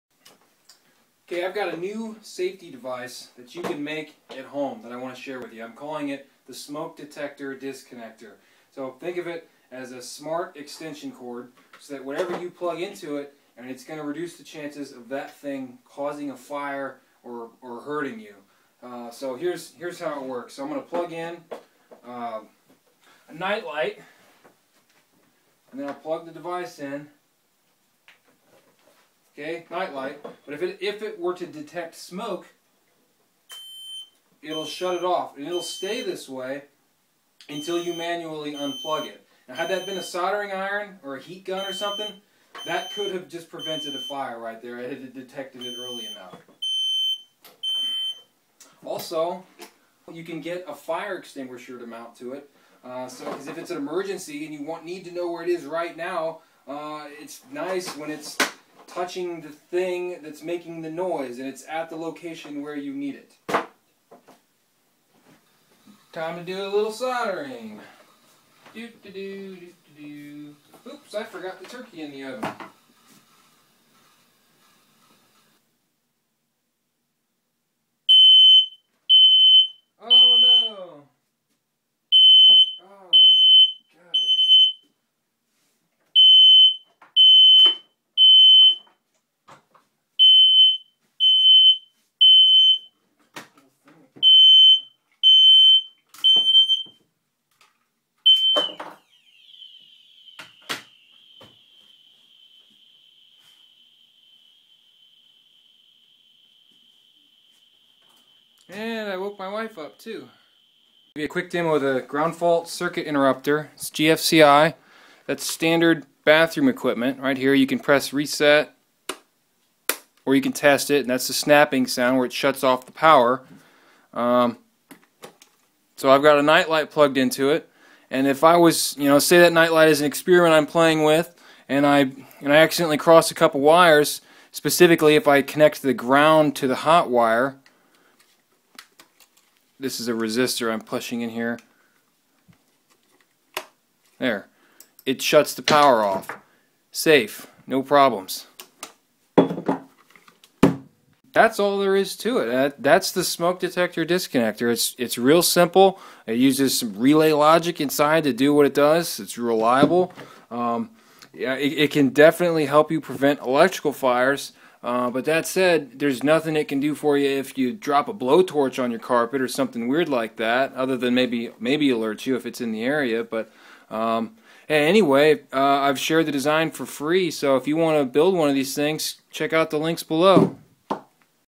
Smoke Detector Disconnector Demo
Think of this as a smart extension cord that reduces the chance of anything plugged into it harming a person or starting a fire.